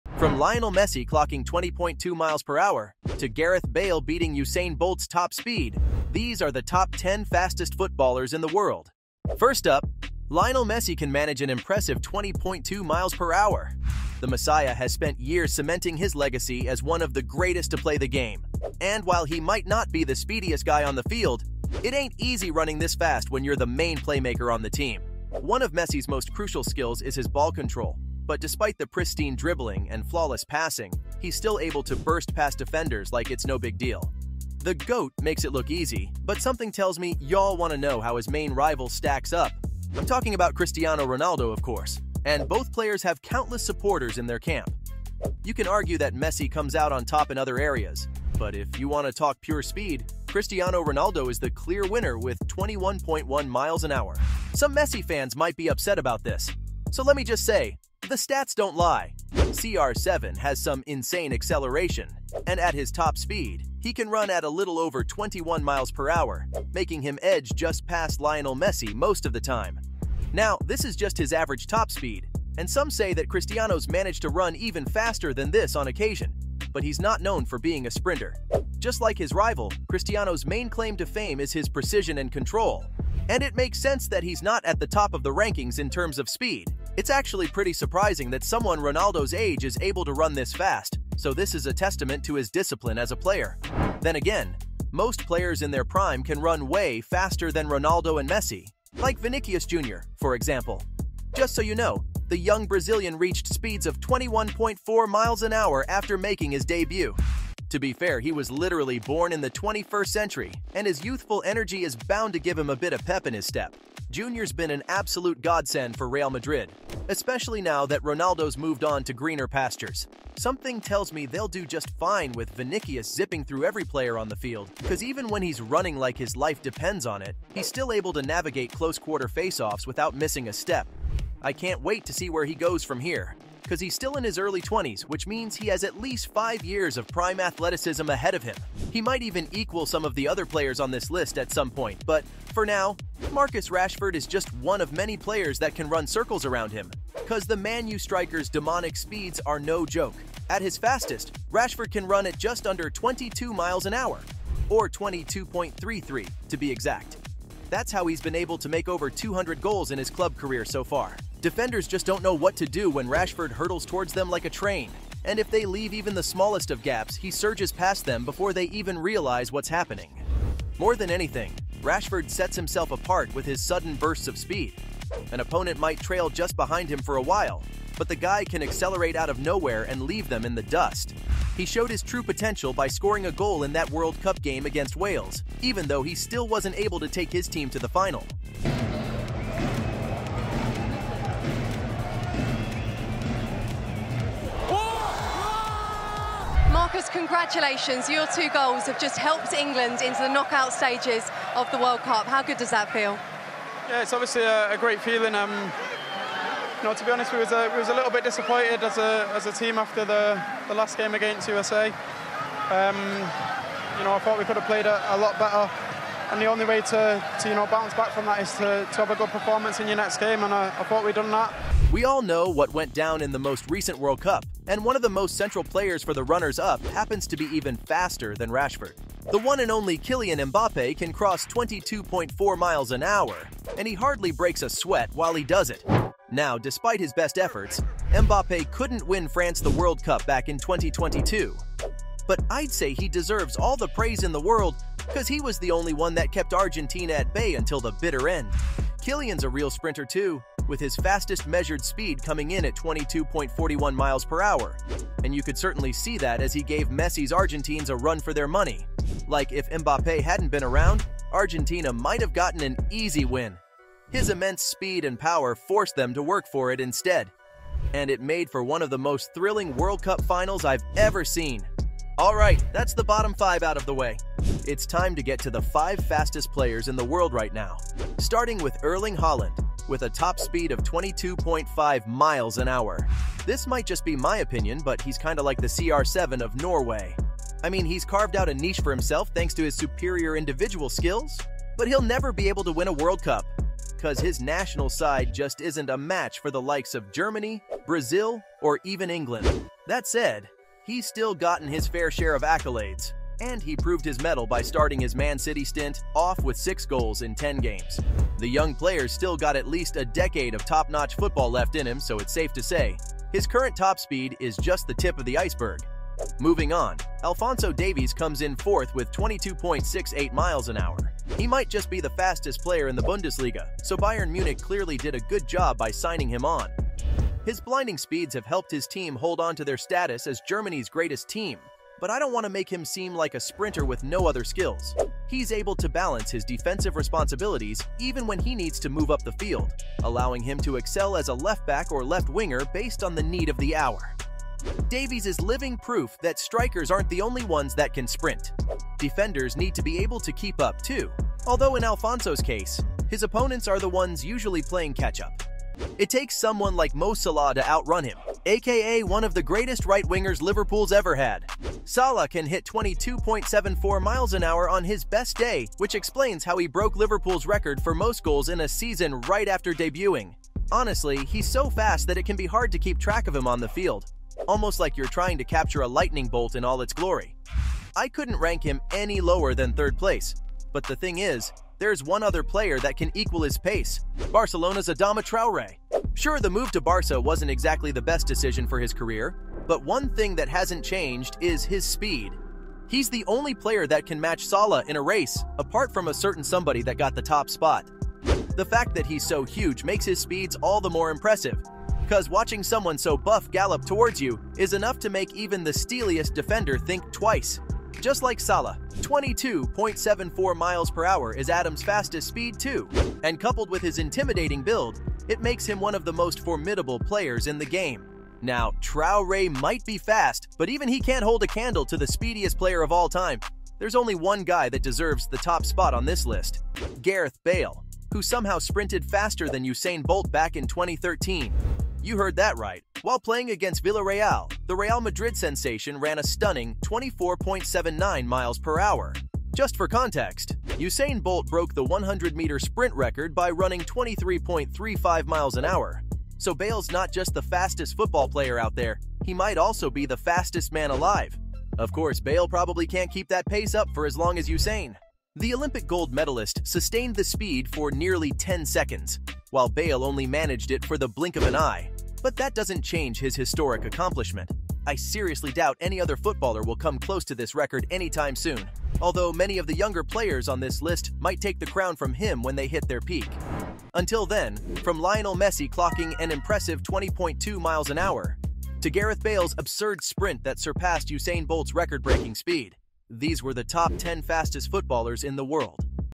Top 10 Fastest Footballers In The World
Welcome back to Flixgoal.Today on the channel we talk about the
fastest football players in the world right now! If you're excited to learn more then make sure you watch the whole way through because you don't want to miss the details we have for you!